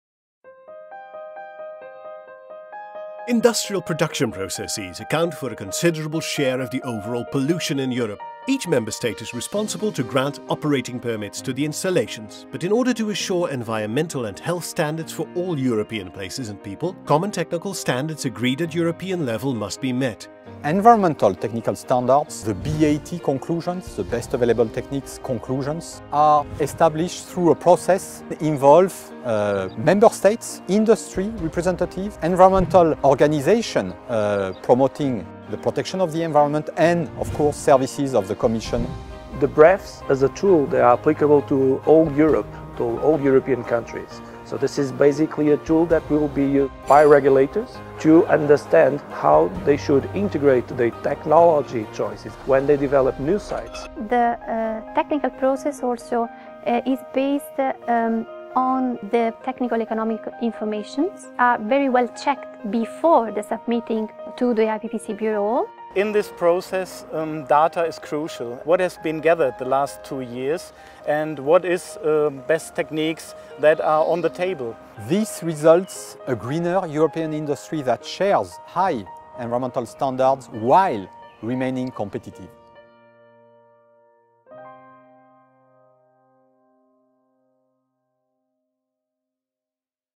Best available techniques to support EU industrial emissions legislation
The European Integrated Pollution Prevention and Control Bureau of the Joint Research Centre produces the Best Available Techniques Reference Documents. These are the scientific and technical basis for EU industrial emissions legislation and are key to the implementation of the Industrial Emissions Directive (2010/75/EU).

50.000 industrial and large scale farming installations across Europe use energy, water and raw materials to operate. As part of its processes, they also produce emissions to water, air and soil. In order to assure environmental and health standards for all European places and people, common technical standards agreed at European level must be met.

Best Available Techniques Reference Documents are the main reference documents used by competent authorities in EU countries when issuing operating permits for industrial installations that represent a significant pollution potential. They inform relevant decision makers about what may be technically and economically available to industry in order to improve their environmental performance and consequently improve the whole environment.